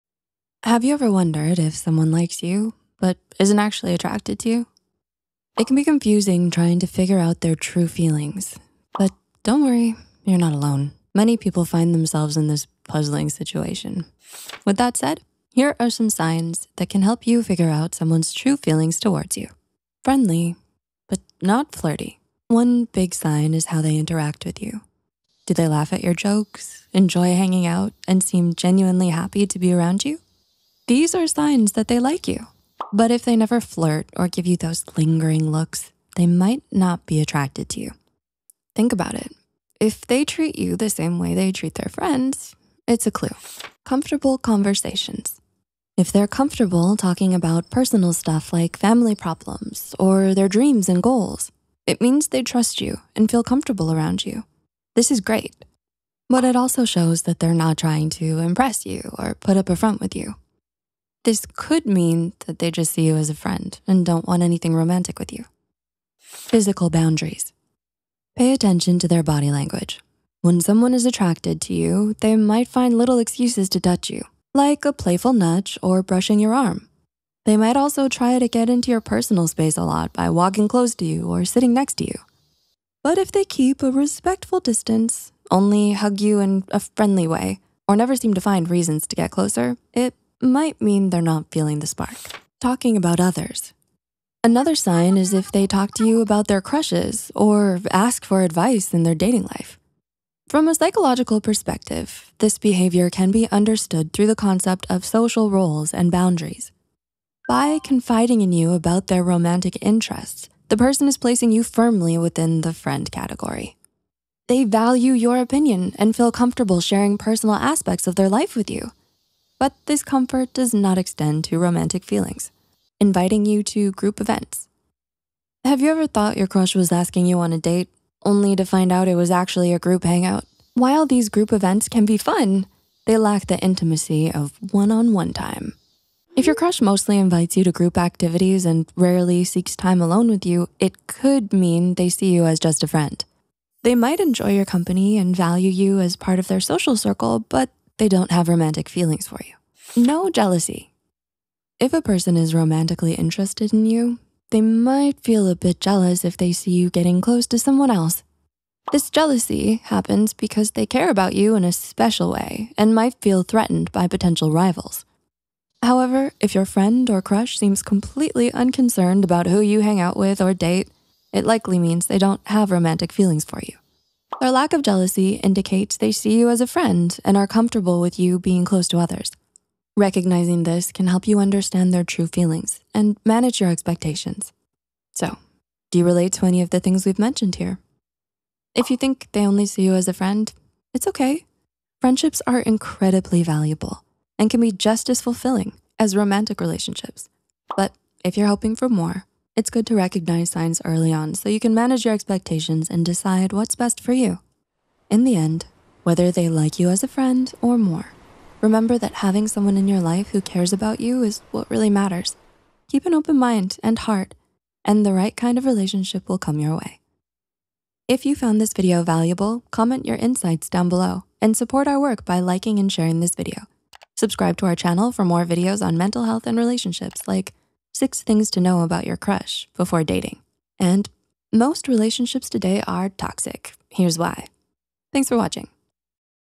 6 Signs Someone Likes You, But NOT Attracted To You
Sometimes, someone may genuinely like you but isn't attracted to you in a romantic way. In this video, we explore six signs that indicate someone enjoys your company and values your connection, but the spark of attraction might not be there. Understanding these subtle cues can help you navigate your relationships with clarity and avoid unnecessary confusion. Whether you're dating or just curious about the dynamics of attraction, this video offers insights to help you better interpret the signals you're receiving.

SEO at the end: “6 Things To Know About Your Crush Before Dating” and “MOST Relationships Today Are TOXIC, Here's Why.”

Writer: Chloe Avenasa
Script Editor & Manager: Kelly Soong
Animator: Naphia
YouTube Manager: Cindy Cheong